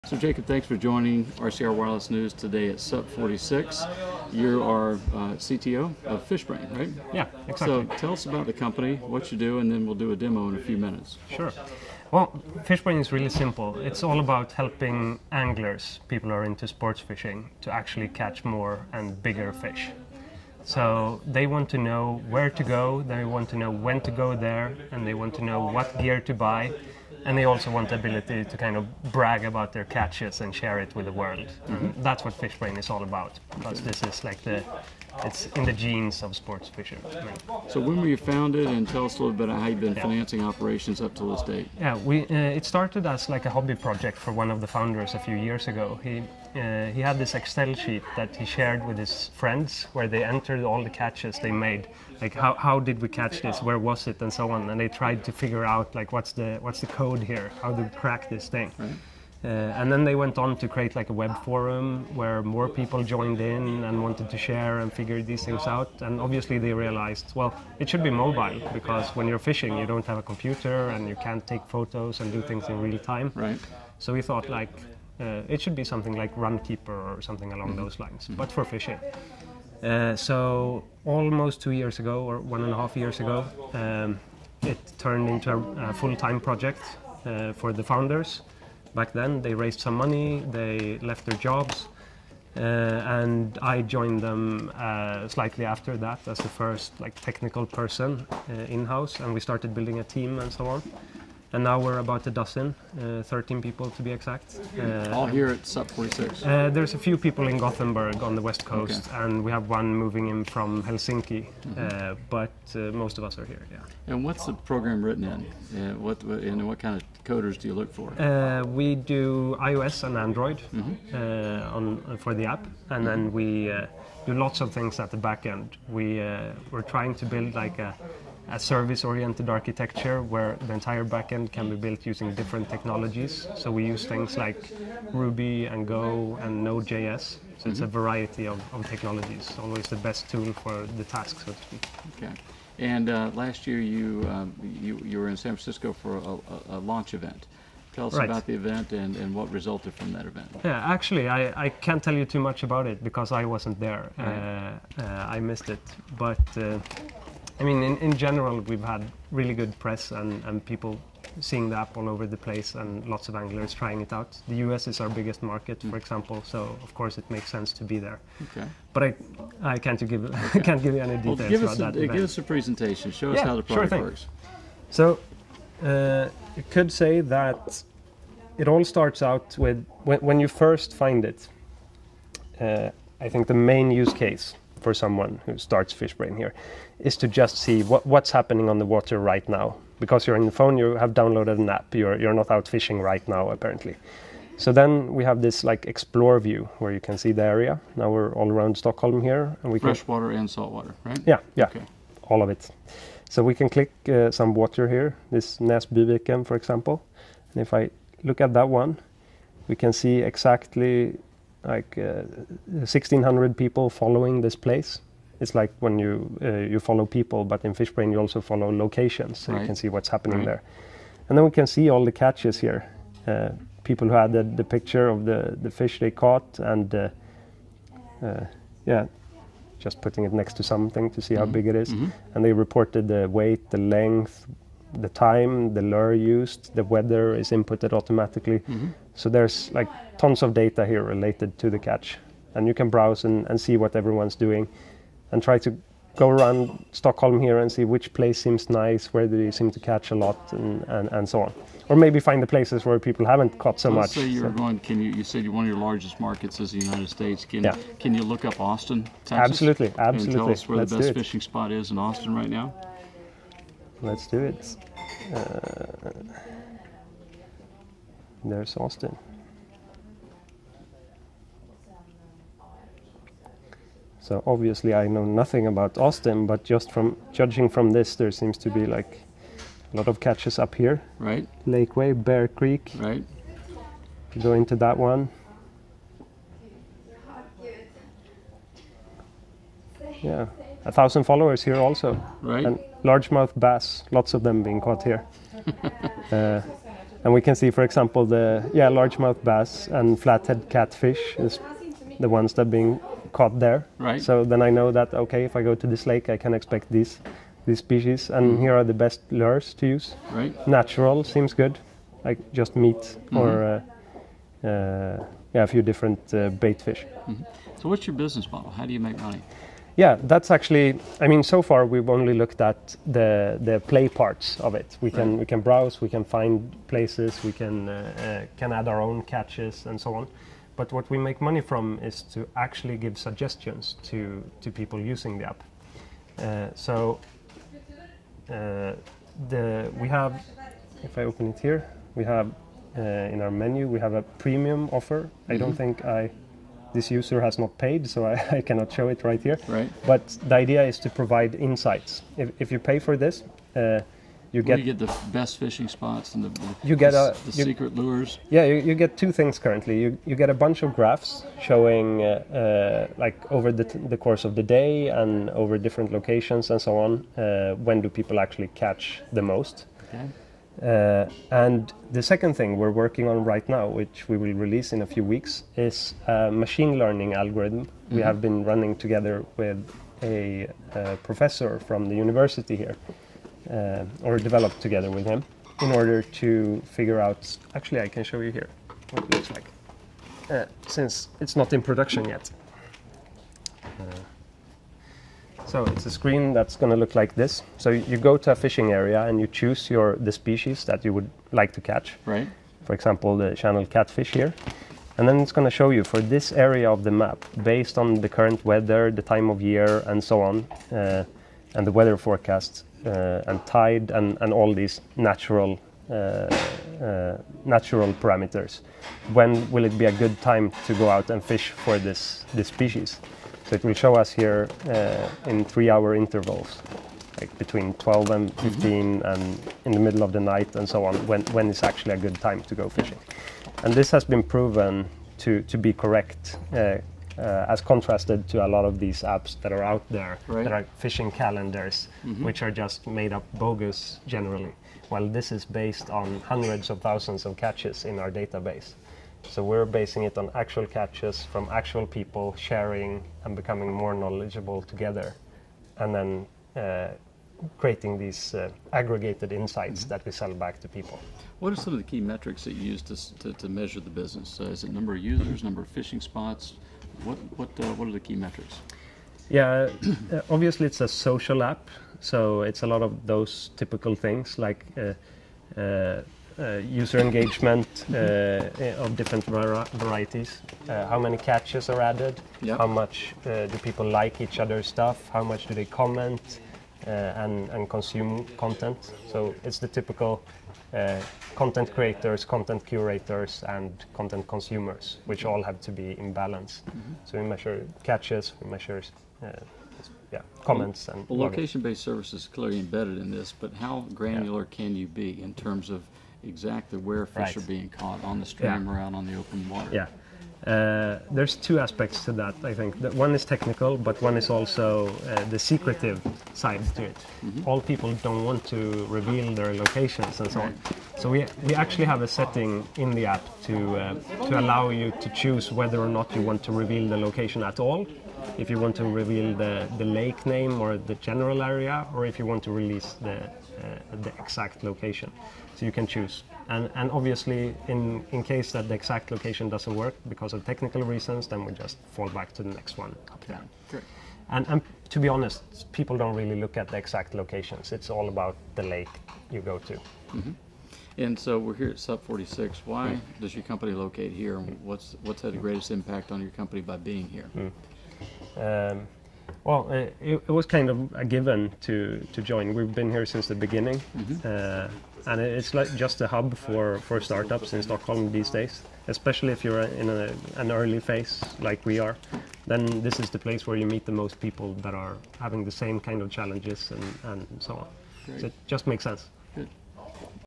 SUP46 Brain Jakob Mattson Demos FISHBRAIN Mobile App
FISHBRAIN's Chief Technology Officer Brian Jakob Mattson talks about the Swedish company's application that allows sports fishermen to share the time, location, equipment setup and other details of their latest catch.  MVI 0284